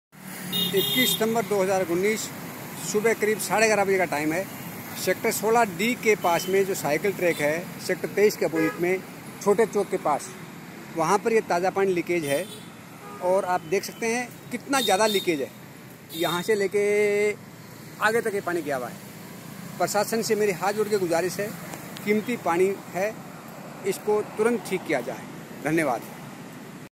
Wastage of water by leakage
यह तो केवल एक सिंपल है इस तरह के रोज शहर में लाखों लीटर पानी बर्बाद हो रहा है लीकेज ठीक ना करने के कारण जिसकी पूरी जिम्मेदारी प्रशासन की है इस पर तुरंत ध्यान देने की आवश्यकता है पर्यावरण बचाने के लिए